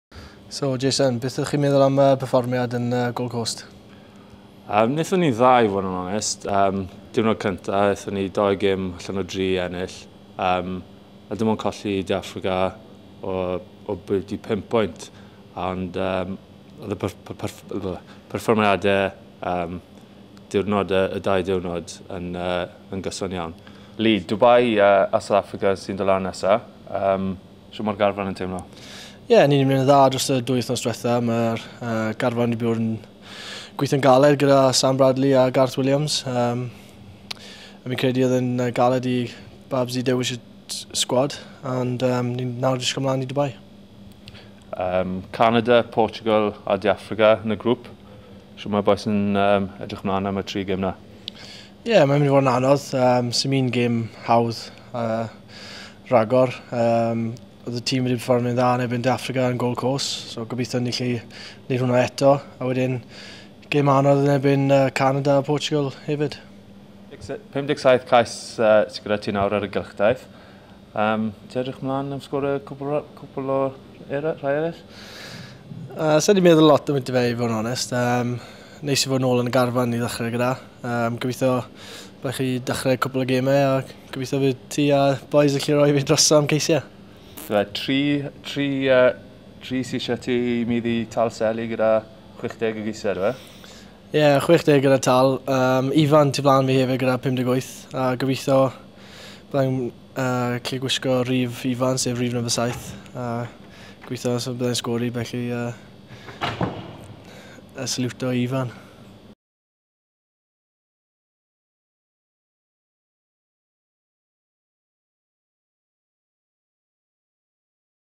Edrych ymlaen at Dubai: Lee Williams a Jason Harries
Lee Williams a Jason Harries o Cwins Caerfyrddin yn edrych ymlaen at gystadleuaeth saith bob ochr Dubai y penwythnos yma, yr ail yng Nghyfres y Byd.